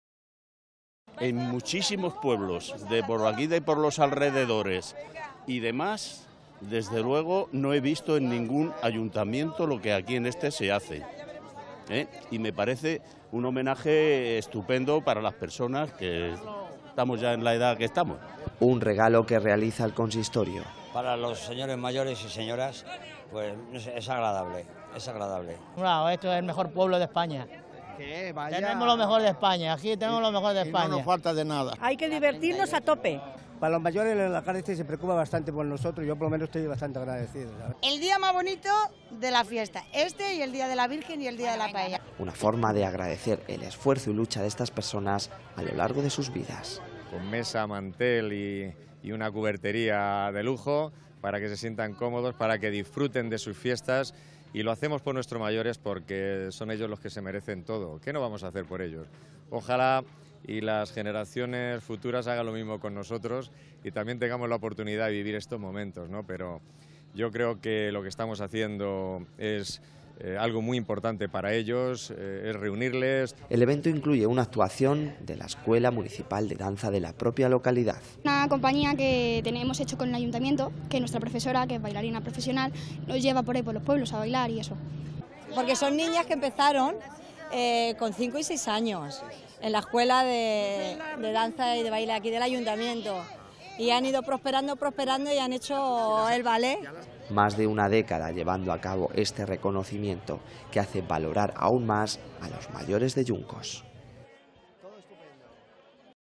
CENA DE GALA EN YUNCOS
No todos los pueblos pueden permitirse obsequiar a sus vecinos más mayores con una Cena de Gala. Yuncos lo lleva haciendo más de una década. El evento incluye una actuación de la Escuela Municipal de Danza de la propia localidad.

Un regalo que realiza el consistorio. Una forma de agradecer el esfuerzo y lucha de estas personas a lo largo de sus vidas.

El evento incluye una actuación de la Escuela Municipal de Danza de la propia localidad.

Más de una década llevando a cabo este recogimiento, que hace valorar aun más, a los mayores de Yuncos.